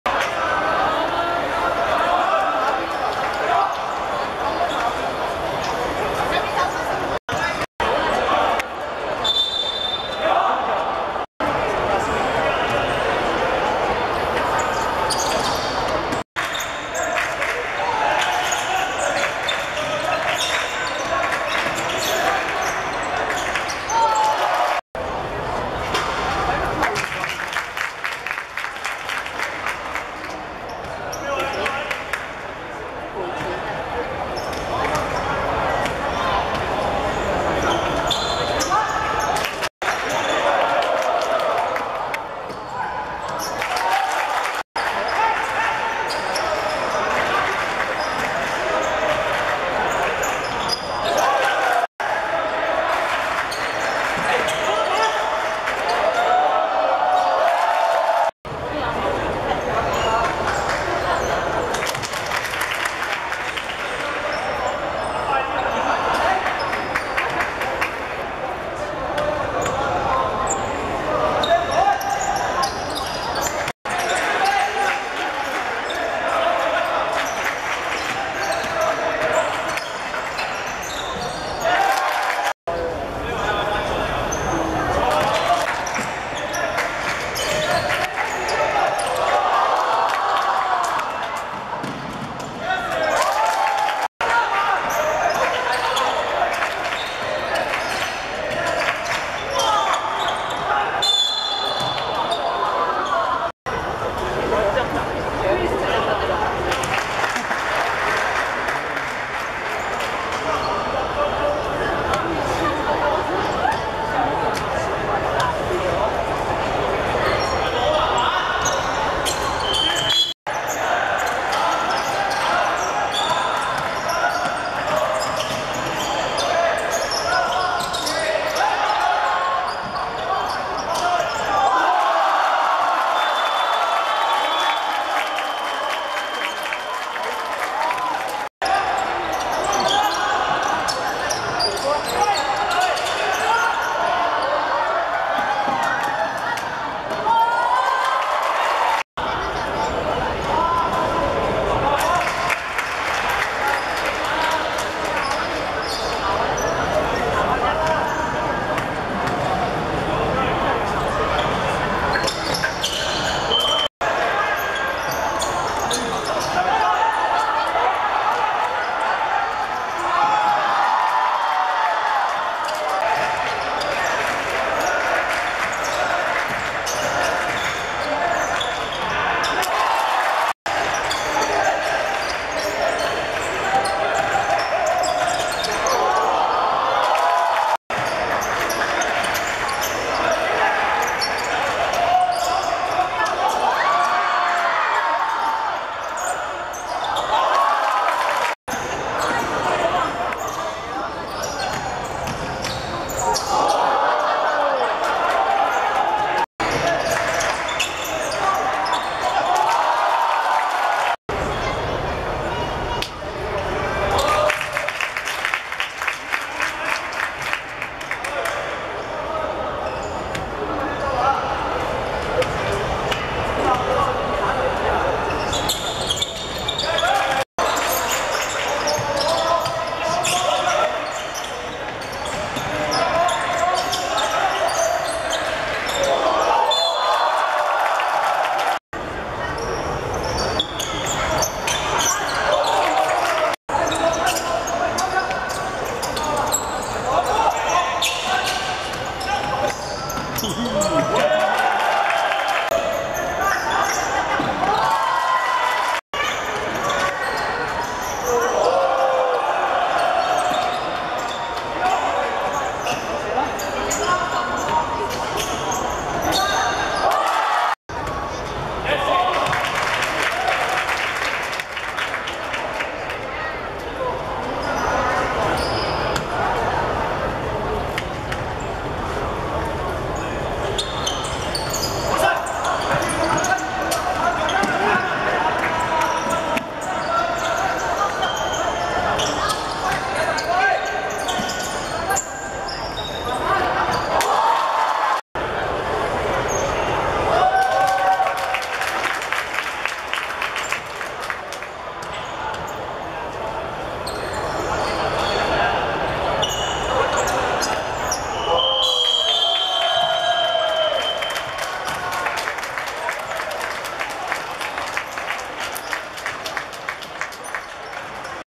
英華vs桂華山(2014.7.6.學界籃球馬拉松~男子決賽)精華
2014.7.6.港灣道體育館 16:55 英華(白) 31:21 桂華山(藍)